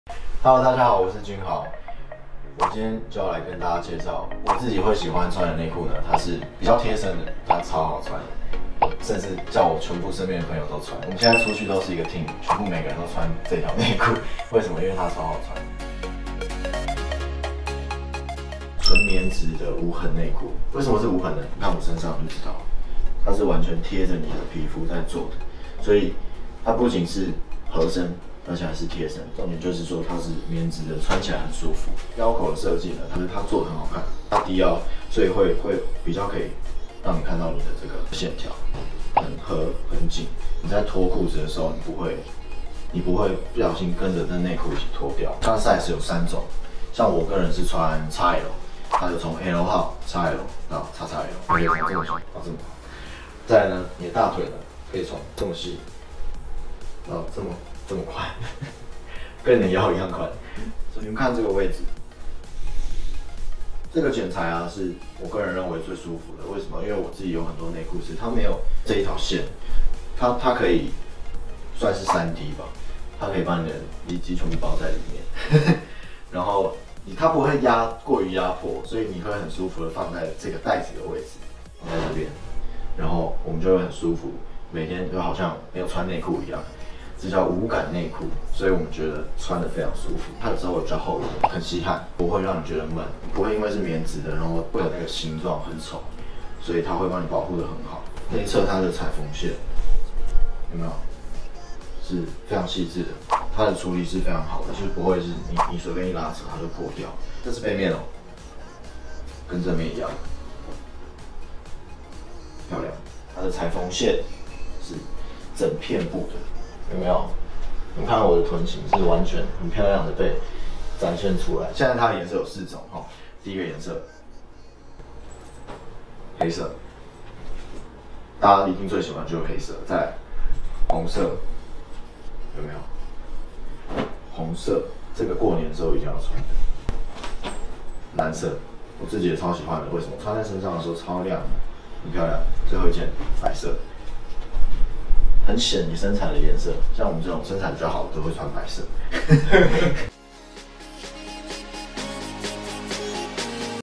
男生內褲推薦哪一款最好穿？讓型男吳君豪親自介紹告訴你
來看這次最新出的內褲，3D包覆內褲！
最特別的是前面剪裁可以裝得下你所有的大小東西😂
雙層太空艙讓你視覺效果放大
而且我一定都是選擇木代爾讓布料更親膚透氣
《木代爾3D包覆無痕四角褲》
重點讓重要部位隨時保持乾爽，透氣舒適不悶熱
顏色就像我影片講的五種顏色
都是很清爽的色系讓你居家只穿一條內褲真的不奇怪
尤其它彈性很好，是採用德國無痕腰帶非常舒服
整件素色一體成形質感真的很好
▪親爽素色 居家穿超適合
▪91%透氣親膚木代爾
▪德國進口無痕腰帶
▪無側縫剪裁 無痕褲腳
▪一片式防夾臀設計
❗總共五種尺寸優惠任3入$579 平均一件$193❗